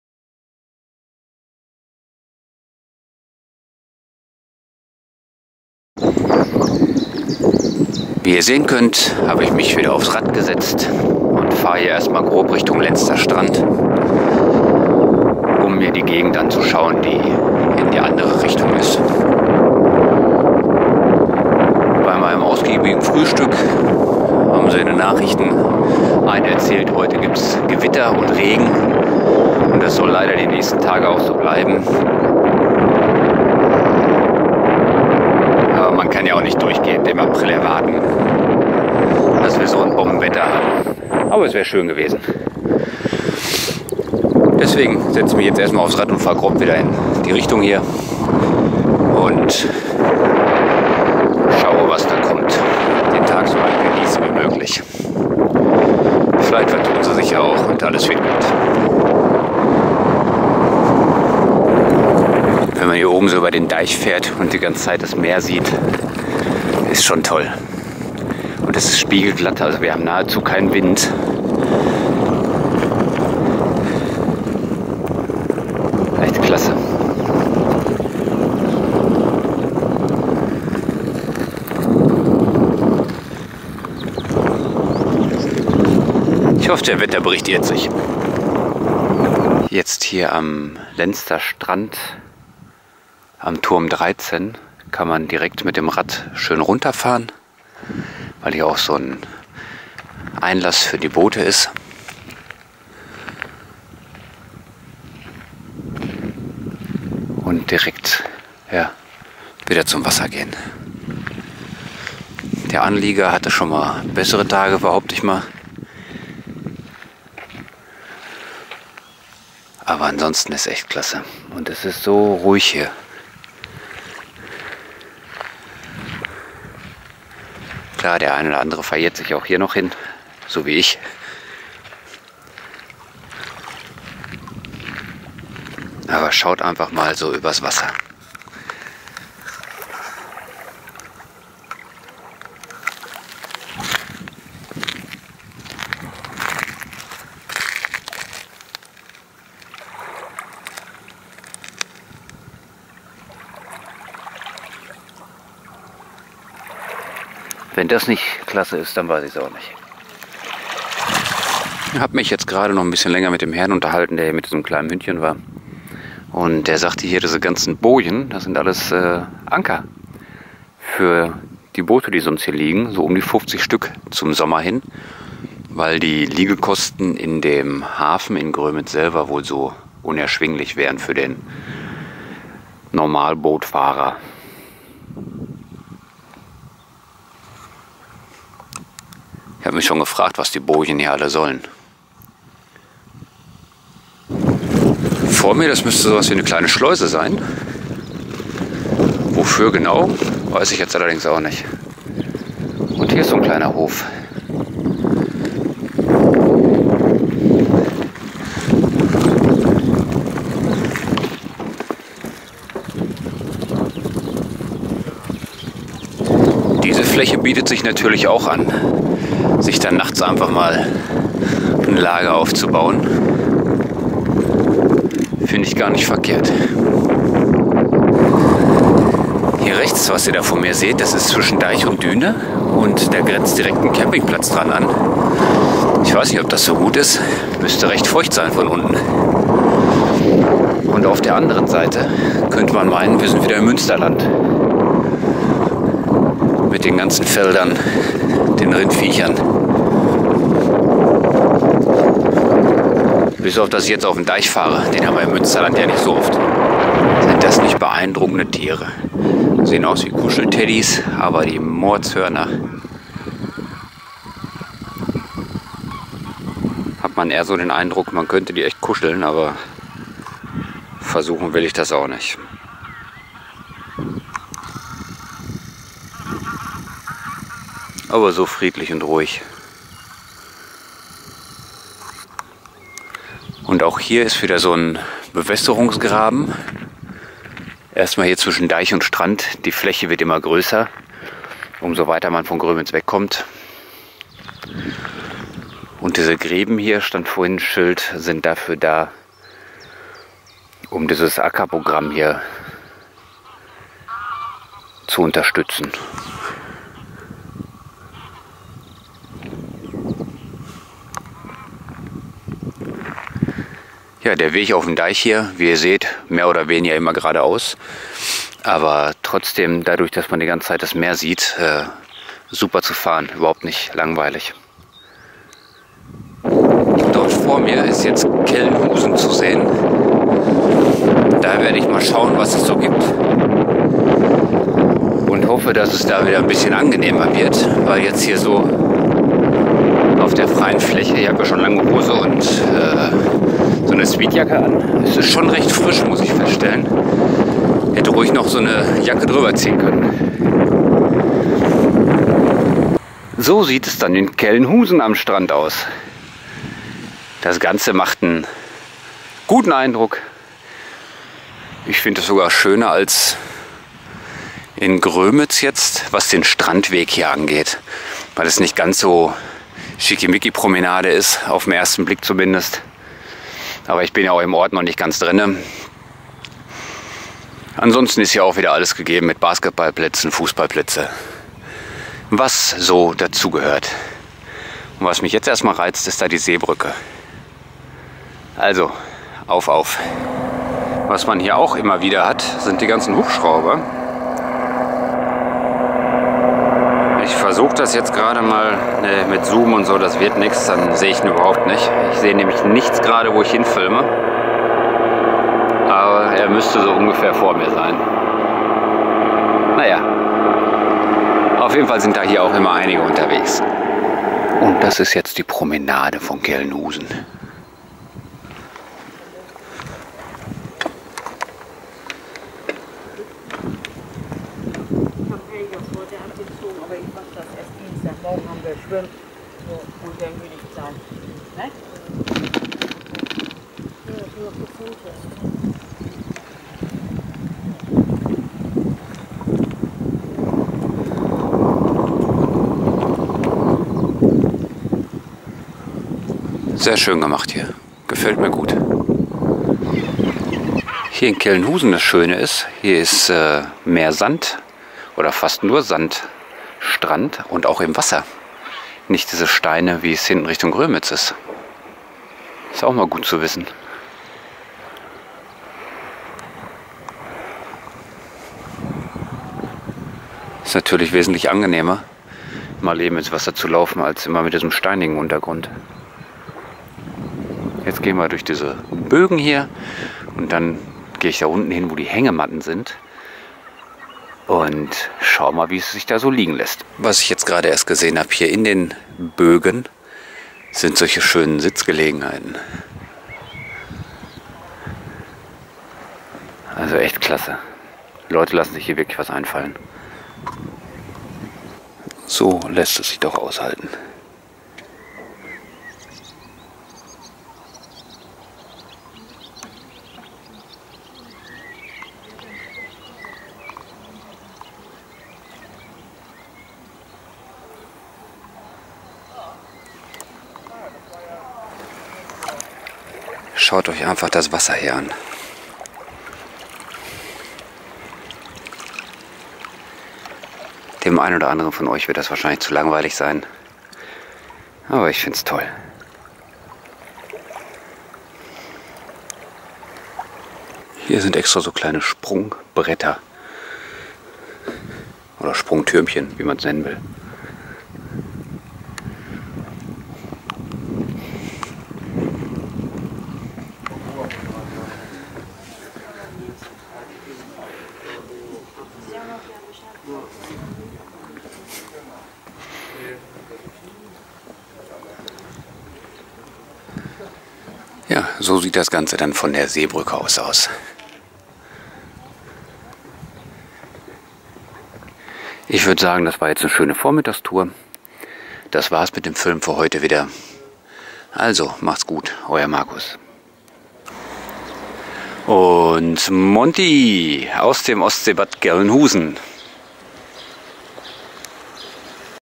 Radtour von Grömitz nach Kellenhusen
Kleine Radtour teils mit starken Windgeräuschen.